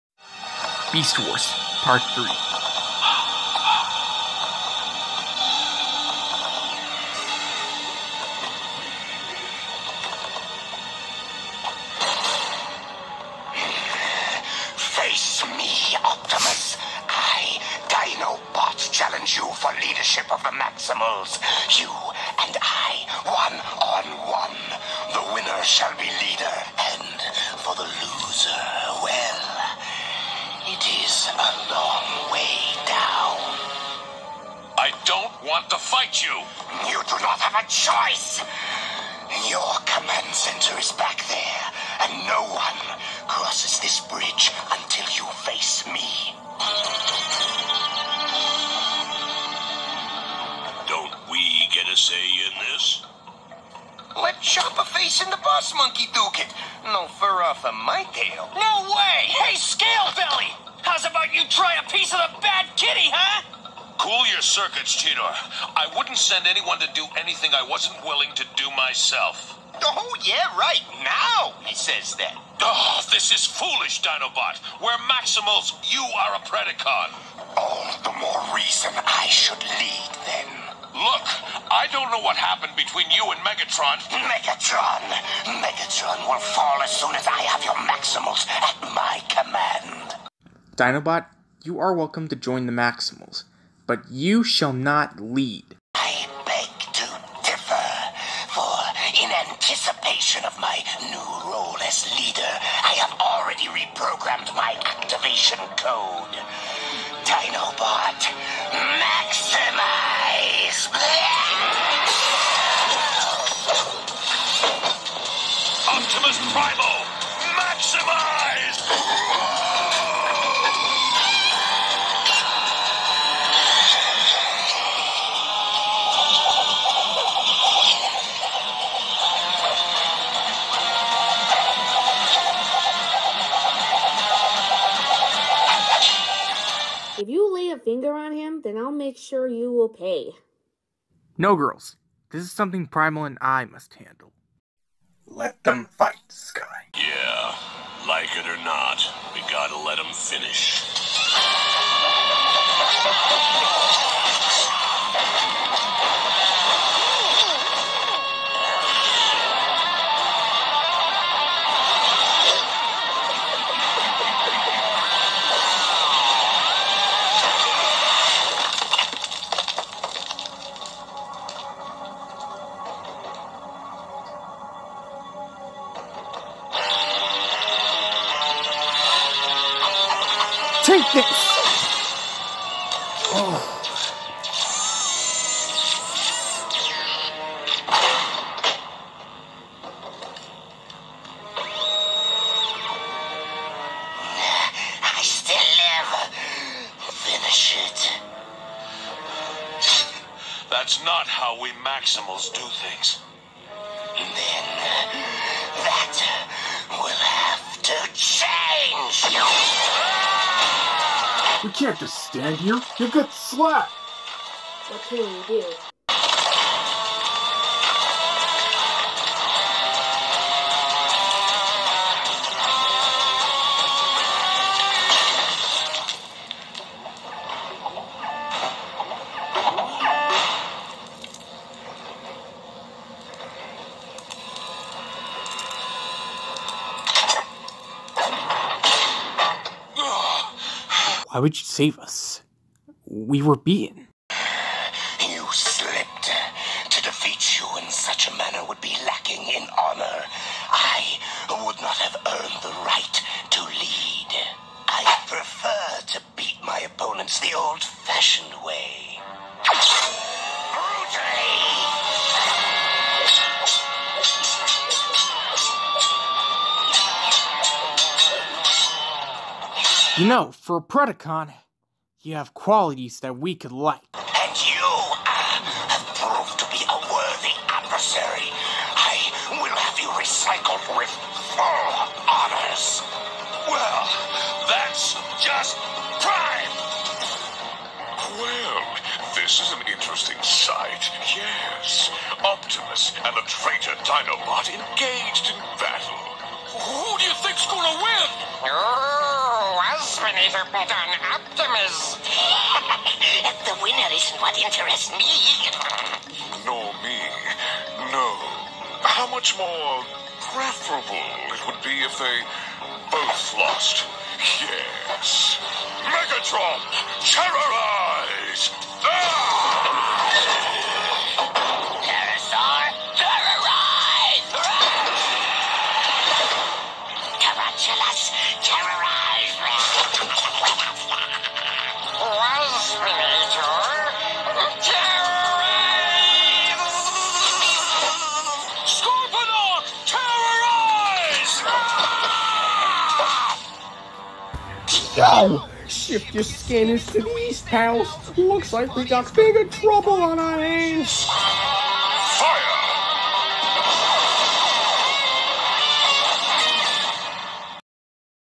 Adventure of Beast Wars part 3.
here is part 3 of Beast Wars. The Fight between Me and Optimus against the defected Dinobot begins. and there is an Unexpected/Unwanted third Group that wants us both gone.